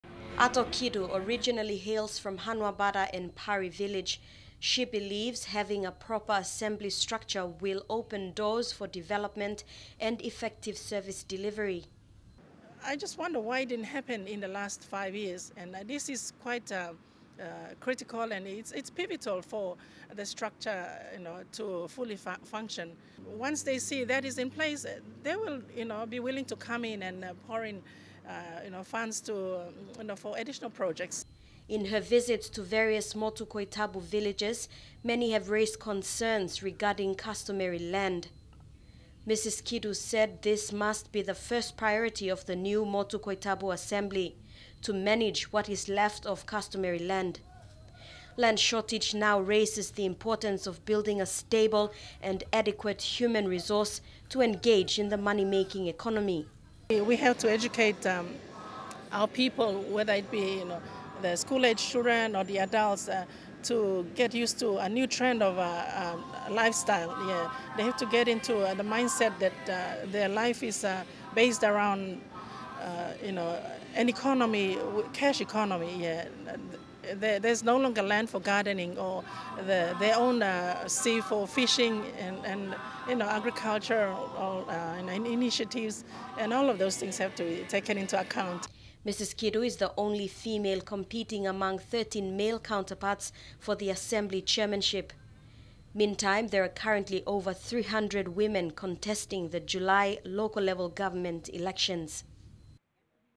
Kidu believes in proper Government Structure
The lone female candidate vying for chairmanship in the Motu Koitabu Assembly elections believes a proper government structure in place will bring about effective service delivery.

Ato Patricia Kidu is among thirteen male candidates contesting the seat.